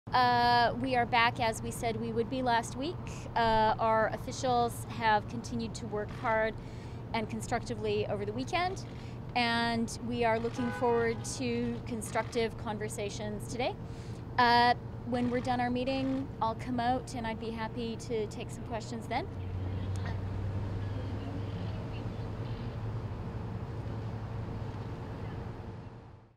Canadian FM Chrystia Freeland: Looking forward to a constructive conversation on NAFTA Talks
Canadian  Foreign Minister Chrystia Freeland said she is looking forward to a constructive conversation with the U.S., before entering the office of USTR Wednesday morning.

Canada and U.S. failed to reach an agreement last week on revising NAFTA. The two countries resumed the talk on Wednesday.